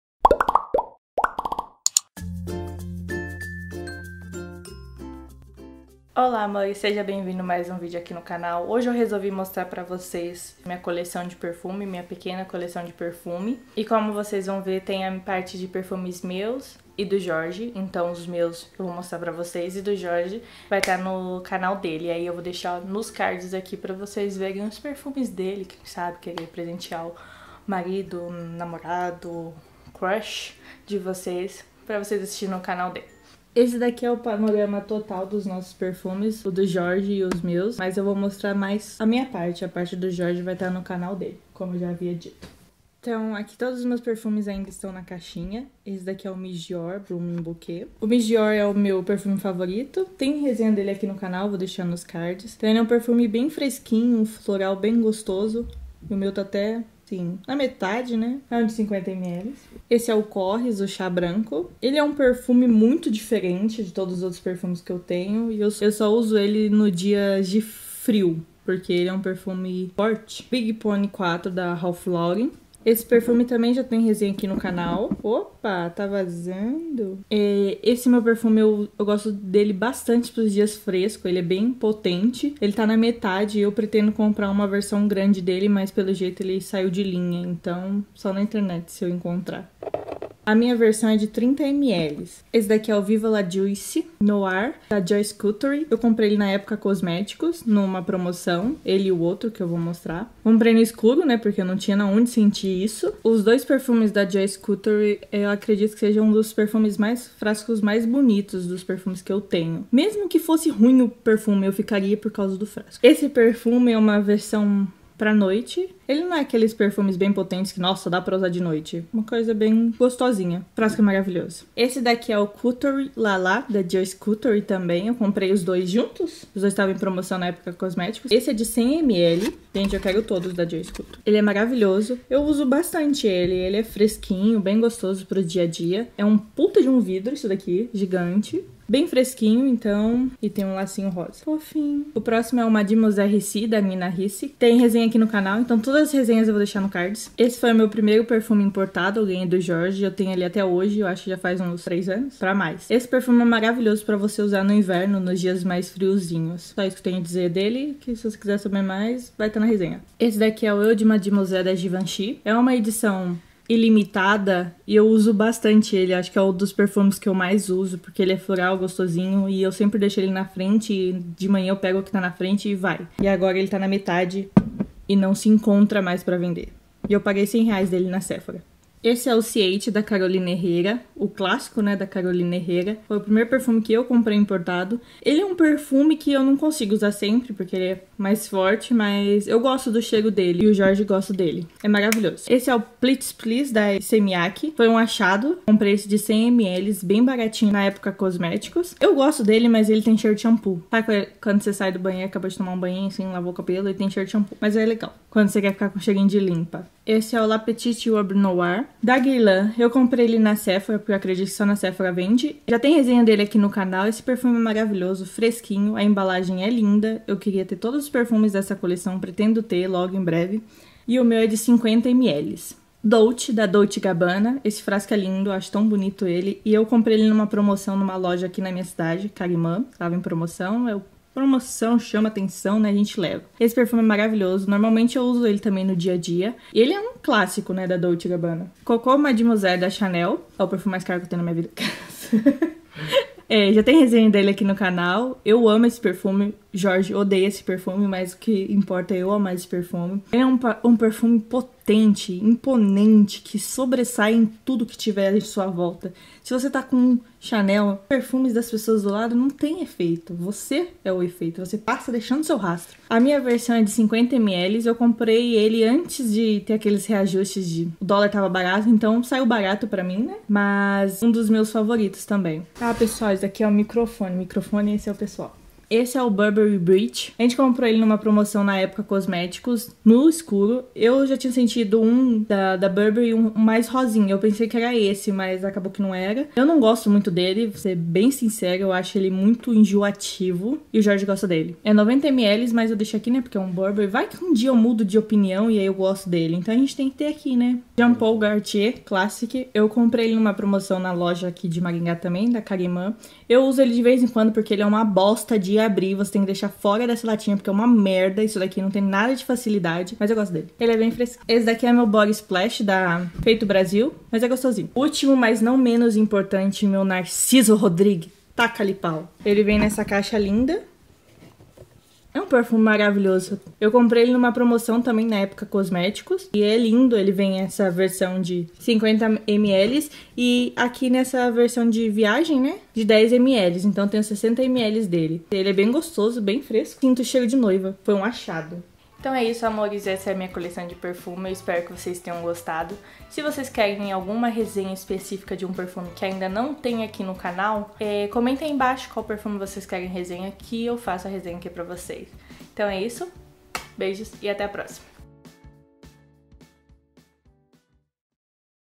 Minha coleção de perfumes 2017 | Ariany Lippi
Olá amores, hoje resolvi mostrar para vocês minha coleção de perfumes. Espero que gostem !!